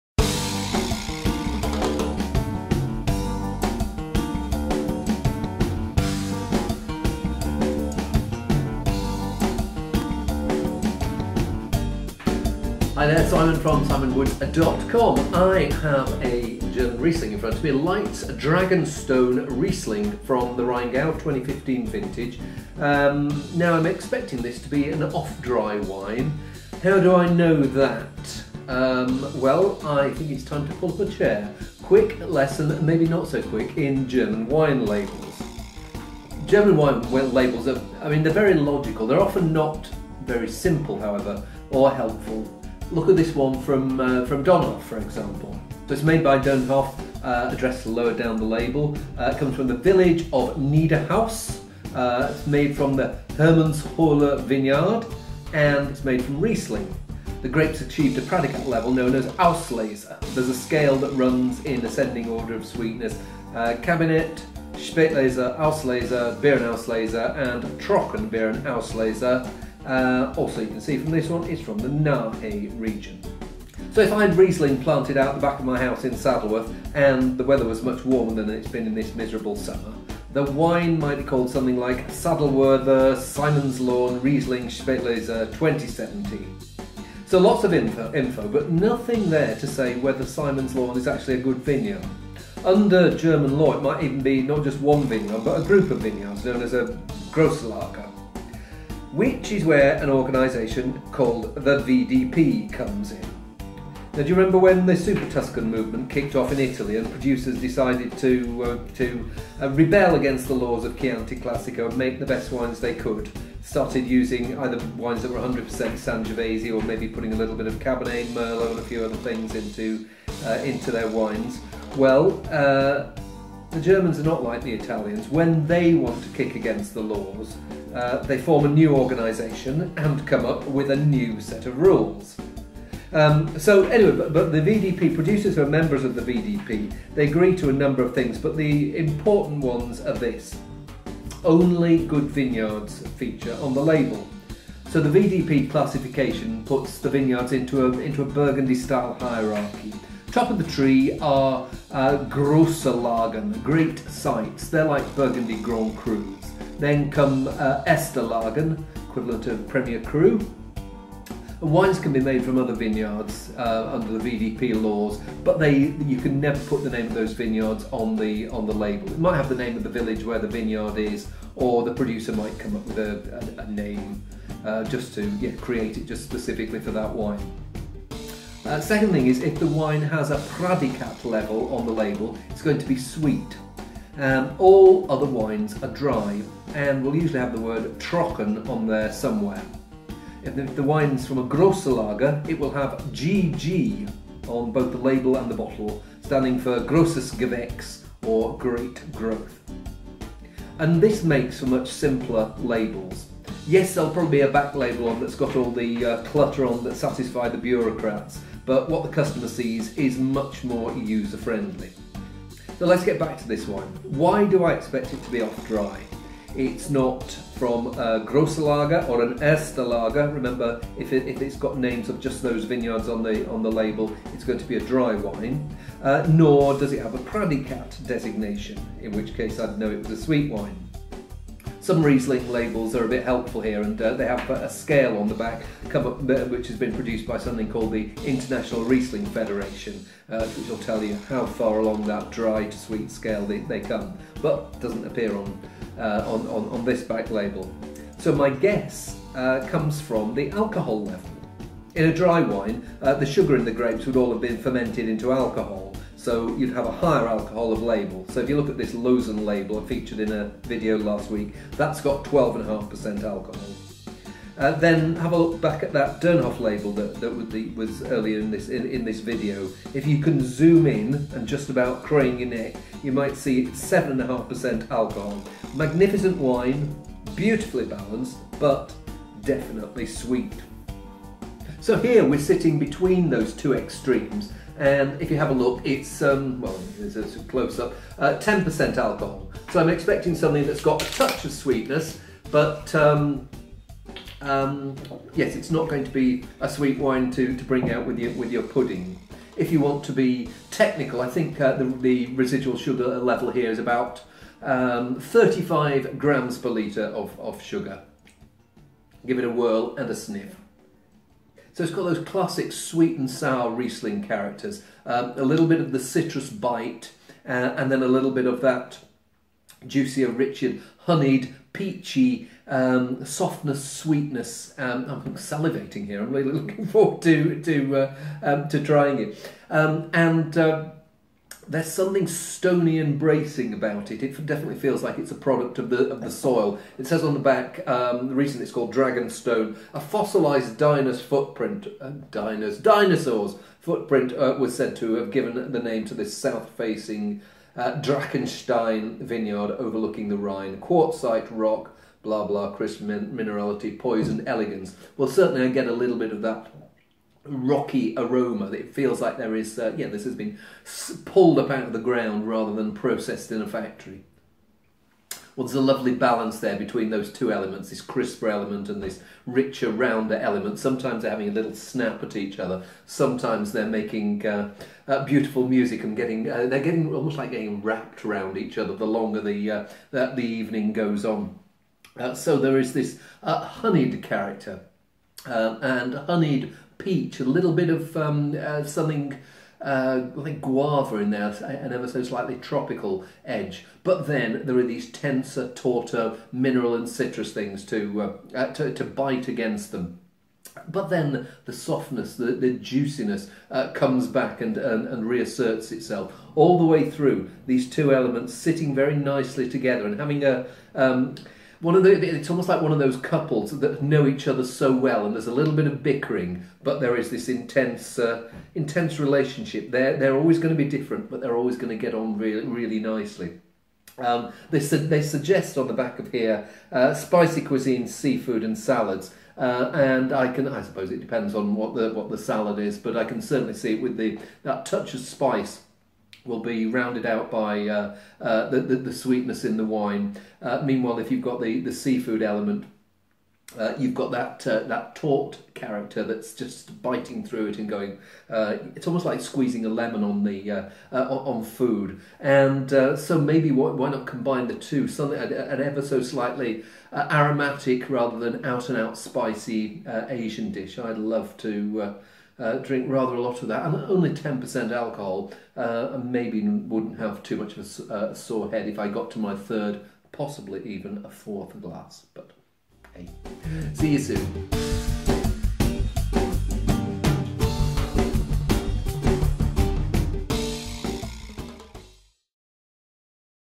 Wine Tasting with Simon Woods: Leitz Dragonstone Riesling 2015, Rheingau, Germany
Simon Woods digs into a a fiery Riesling. Or is it...?

Leitz Dragonstone Riesling 2015, Rheingau, Germany (£13.99 Ake & Humphris, Asda Online)